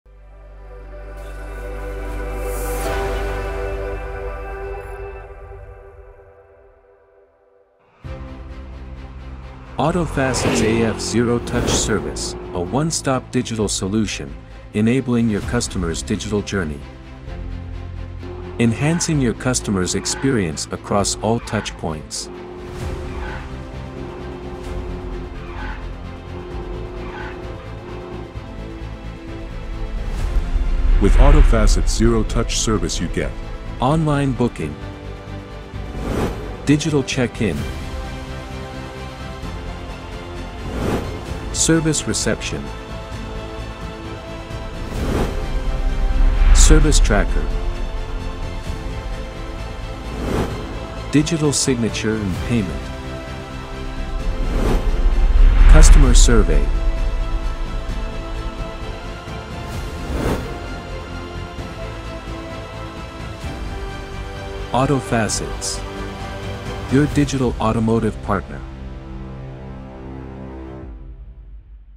AF-X Zero Touch Services | Digital Aftersales Journey
Get ready to enhance your customers' digital aftersales experience with AutoFacets.

Embark them on a digital journey with AF Zero Touch Services, with customised workflows and faster integration, resulting in higher customer satisfaction.